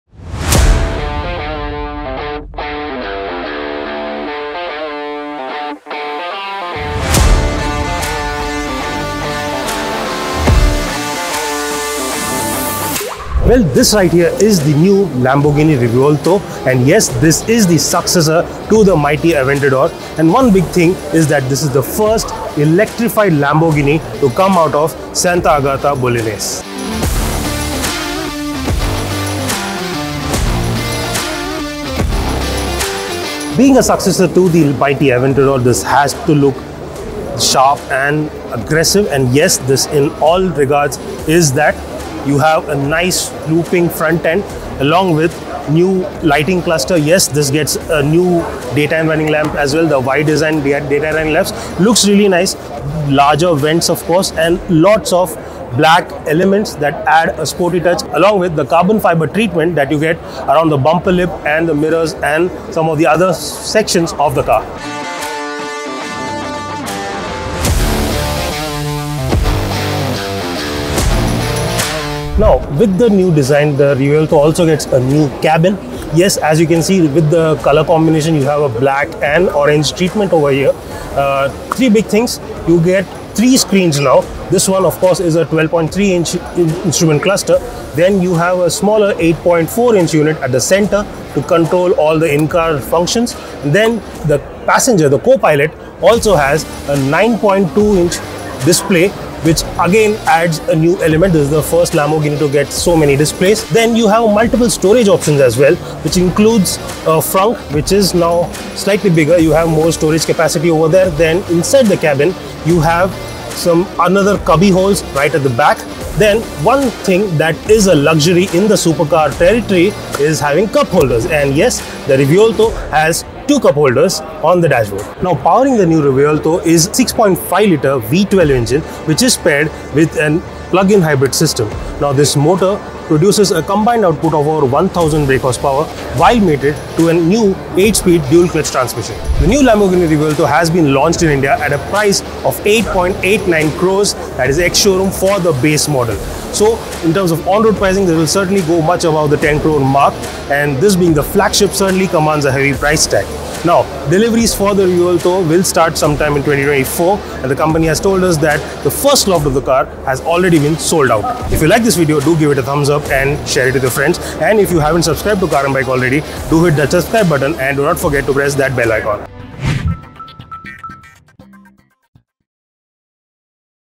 Lamborghini Revuelto Launched In India | Rs. 8.89 Crore For This Electrified V12 Supercar
Lamborghini has finally launched the much-anticipated Revuelto in India. In addition to being a successor to the mighty Aventador, the new Revuelto is also the first high-performance plug-in hybrid to come out of Sant'Agata Bolognese. This new supercar comes with the new avatar of the iconic V12 engine, which is now paired with a plug-in hybrid system with three electric motors, offering a combined output of 1001 bhp and 725 Nm. It’s also paired with a new 8-speed DCT unit. Priced at Rs. 8.89 crore (ex-showroom, India), deliveries for the Lamborghini Revuelto will begin sometime in 2024, however, the first lot has already been sold out.MORE FROM C&B